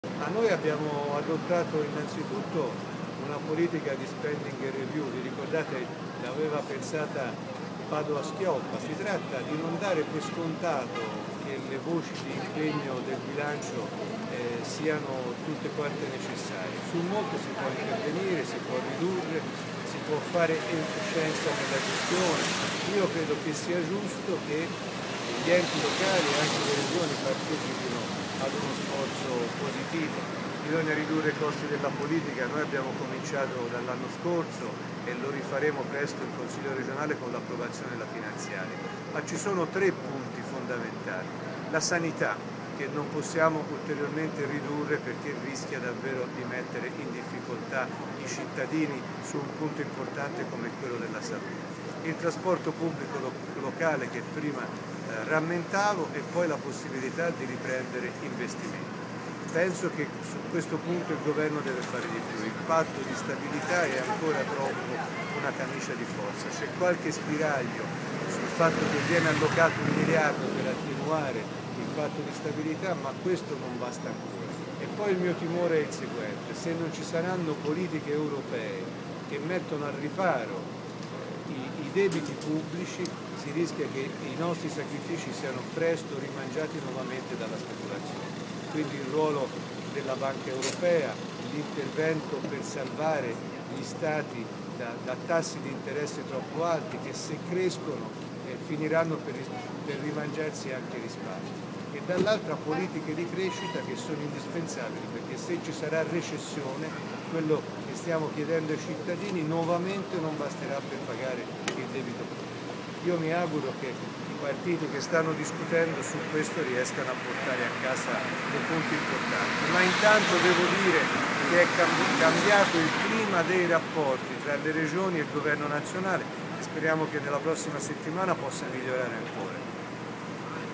Enrico Rossi, a SkyTg24: "Servono politiche per la crescita, altrimenti ogni sforzo sarà inutile"
Intervista a SkyTg24 ore 13.00 di sabato 10 dicembre 2011. (Seconda parte)